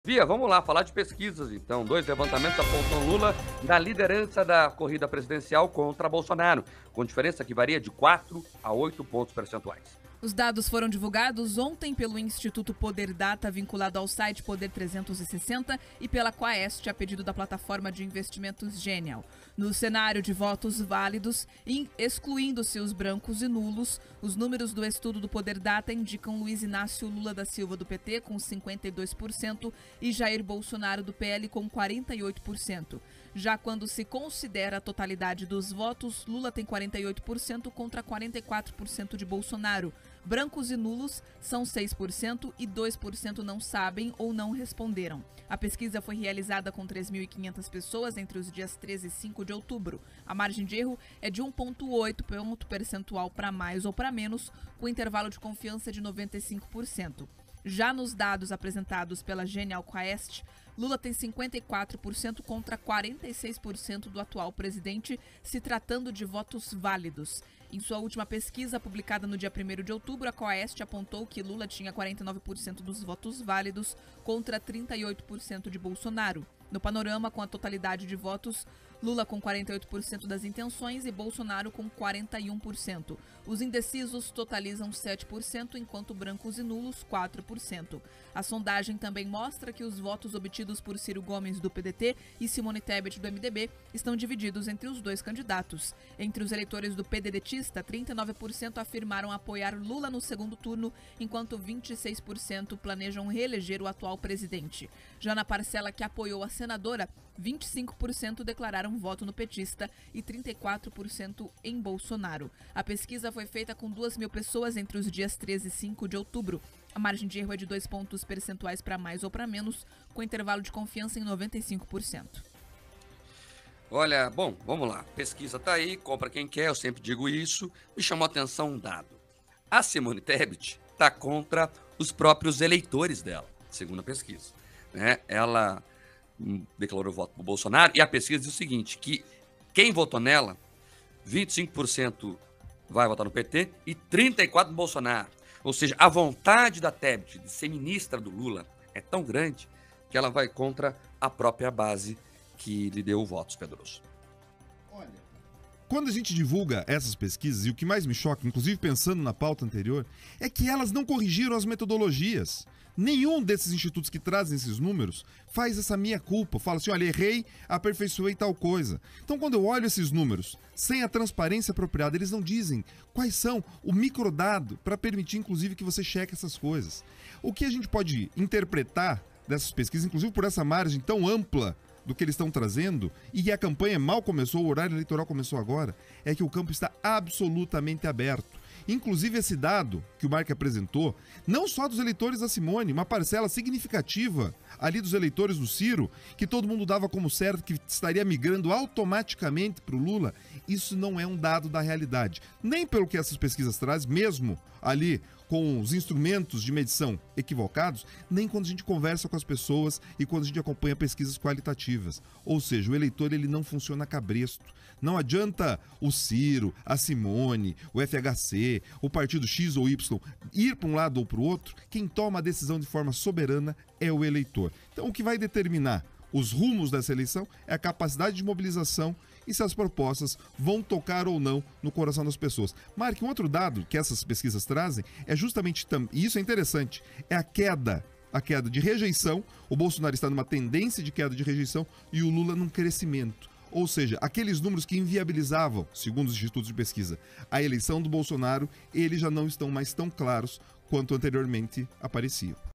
Duas novas pesquisas de segundo turno
Dois novos levantamentos referentes às eleições apontam Luiz Inácio Lula da Silva na liderança da corrida presidencial.